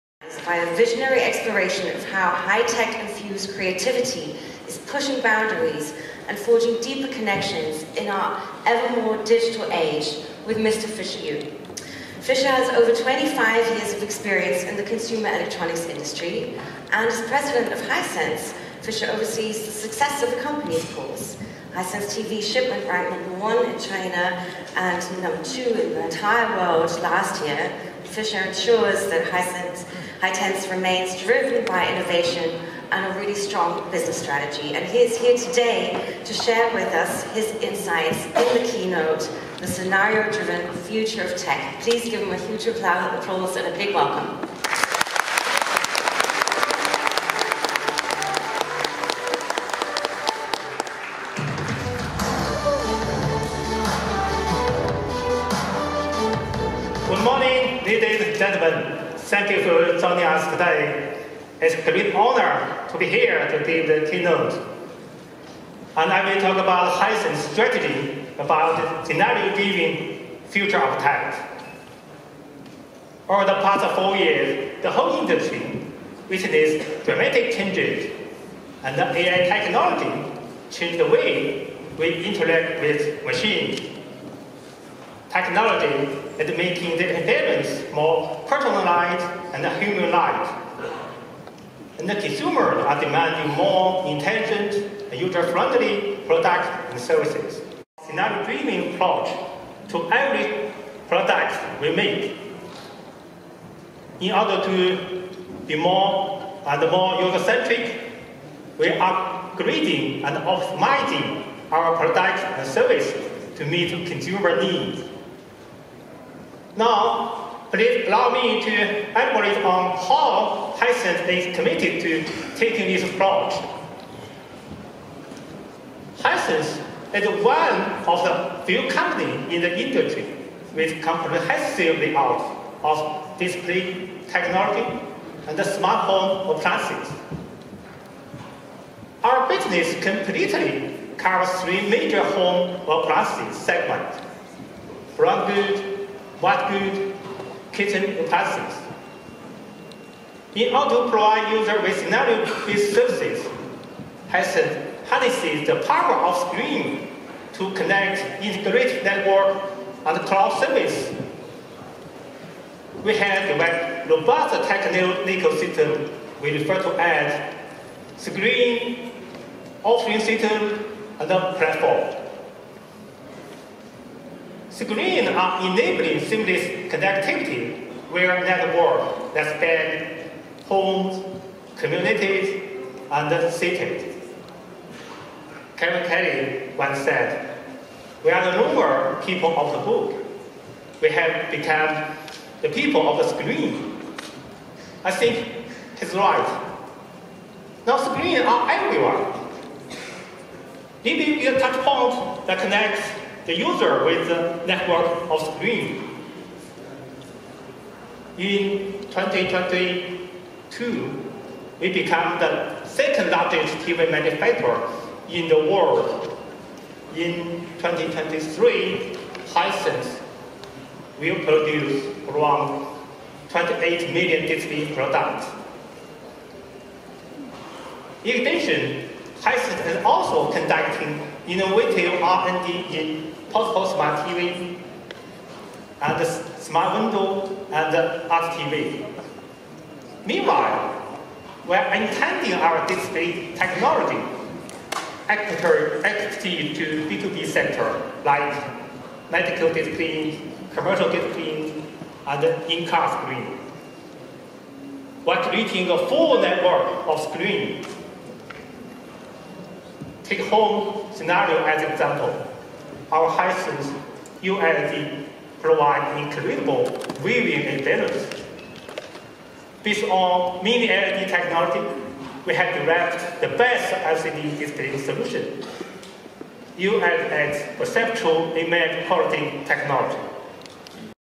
Hisense IFA 2023 Fisher Yu - President of Hisense about Future of Tech
Global consumer electronics and appliances corporation Hisense as IFA Lead Partner made a keynote debut today at IFA 2023, where Fisher Yu, President of Hisense Group Holdings Co., Ltd. delivered a speech on the theme of "The Scenario-driven Future of Tech".